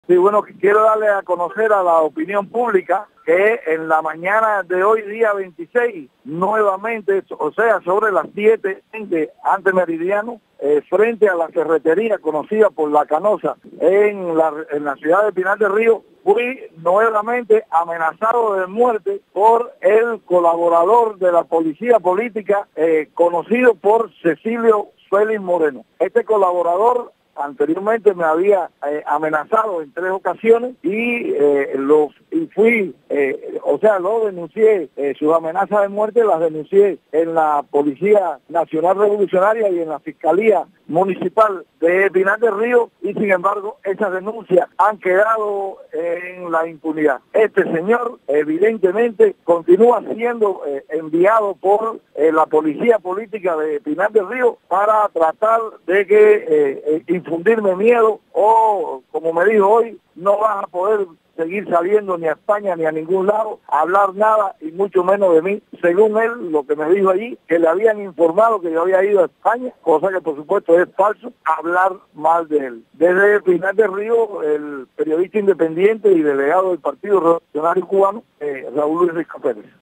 Amenazan de muerte a opositor cubano Raúl Risco Pérez
Raúl Luis Risco Pérez, periodista independiente y delegado del Partido Revolucionario Cubano desde Pinar del Río denuncia que fue amenazado de muerte nuevamente por el colaborador de la Seguridad del Estado Cecilio Félix Moreno, " Este colaborador anteriormente me había amenazado en tres ocasiones y sus amenazas de muerte las denuncié en la Policía Nacional Revolucionaria y en la Fiscalía Municipal de Pinar del Río y sin embargo esas denuncias han quedado en la impunidad"